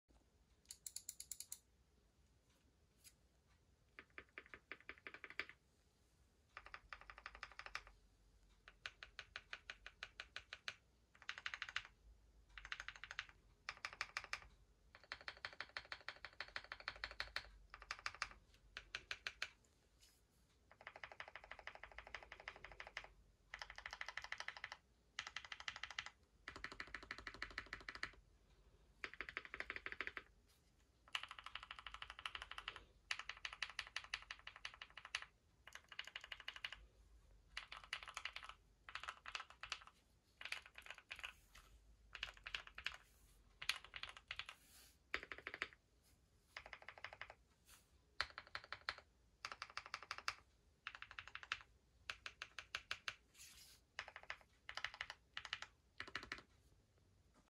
Updated my Bakeneko60 + Zaku Switches
Was using durock v2 stabs on the backspace and spacebar which was pinching the o-ring before. Now im using the clip in stabs that came with the kit for the backspace and spacebar.

Case: Bakeneko60
Plate:Fr4
Switches: Zakus L + F w/krytox 205g0 and deskeys film
Stabilizers: durock v2 and Clip in stabs that came with the kit for the spacebar and backspace
Keycaps: Gmk Godspeed Columbia